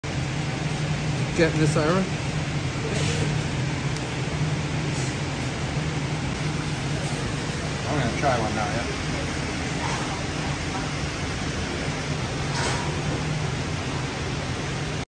Crazy BBQ Mushroom Briskets The Size of My Head
Full episode scenes from cooking for an all meat festival as vegan chefs